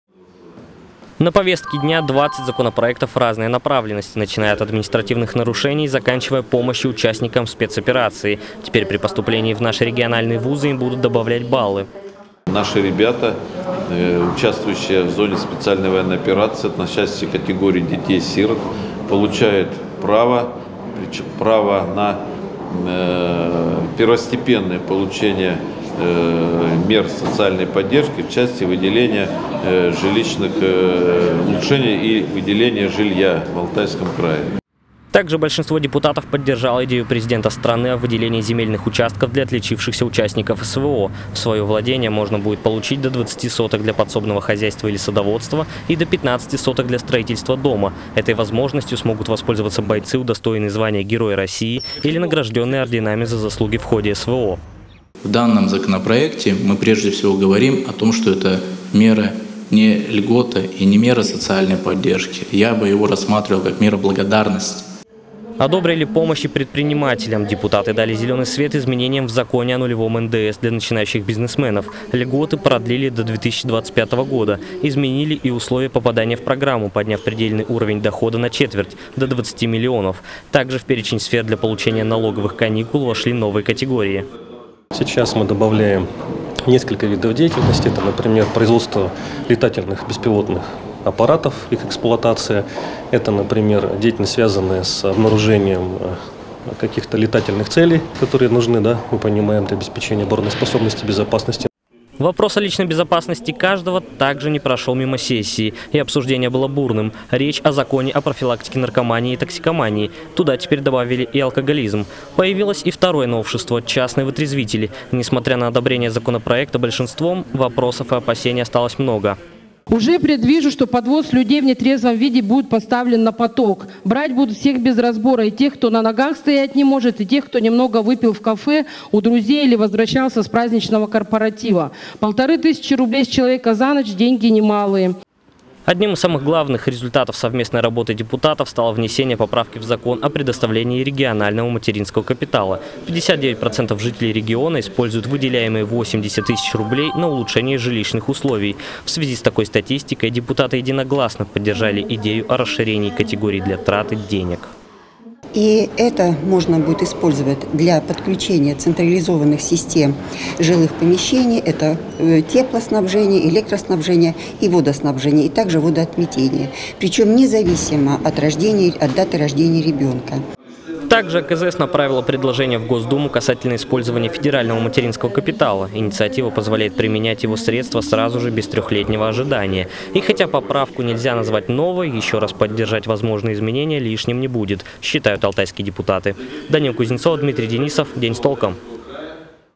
Депутаты АКЗС вышли с каникул и сразу обсудили несколько важных законопроектов
Прежде всего поддержали инициативы губернатора, устанавливающие гарантии для участников СВО. Рассказываем, что еще обсудили на сессии.